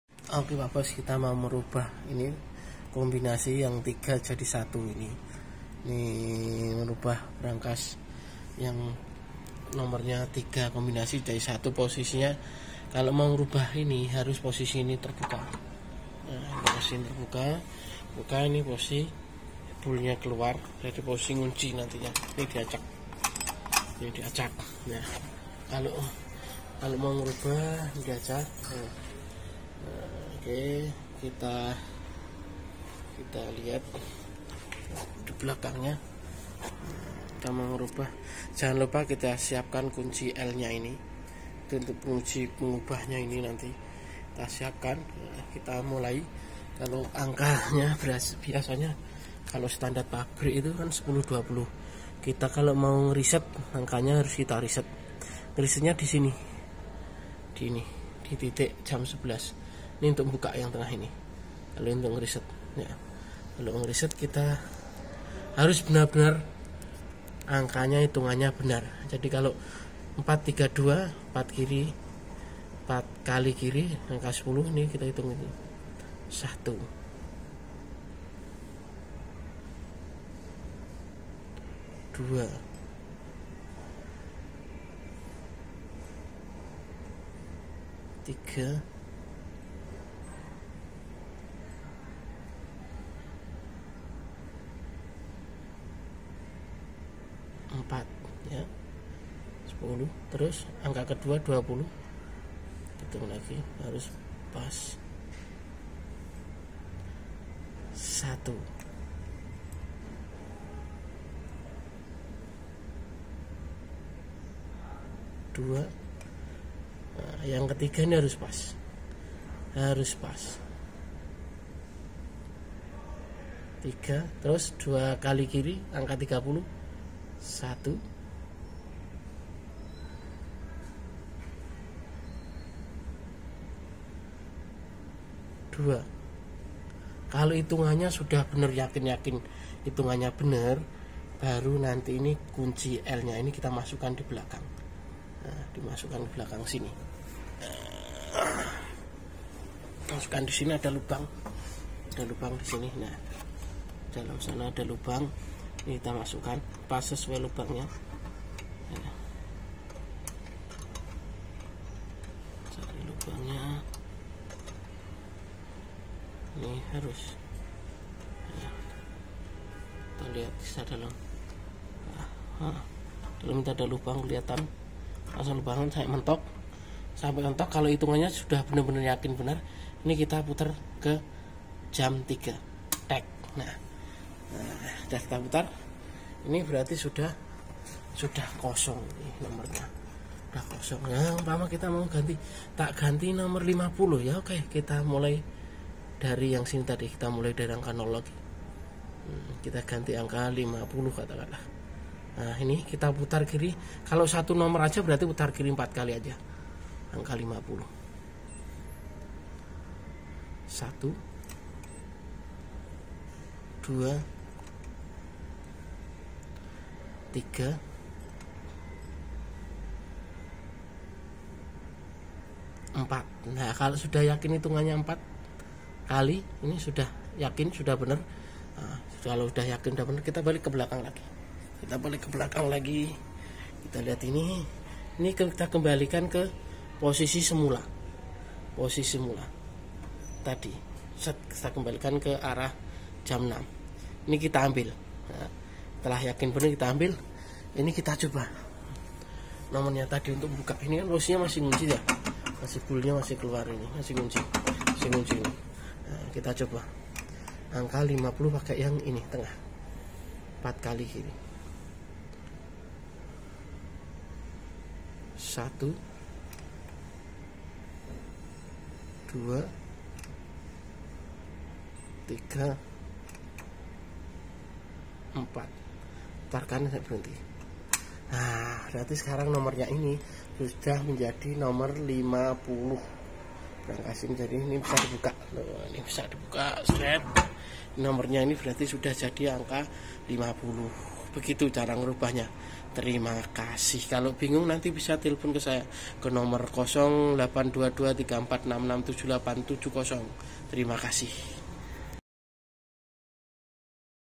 Cara mudah merubah kombinasi Brankas 3 nomer menjadi 1 nomer monggo dipahami bingung telpon saya Bos
Kami distributor Brankas telpon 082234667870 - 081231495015 show room kami di maspion square JL. A. Yani No, 73 surabaya lantai dasar blok B 19-20
vidio ini adalah cara mengganti nomor kombinasi brankas 3 nomer jadi 1 nomer agar mudah diingat Boss... kalau bingung telpon atau wa saya boss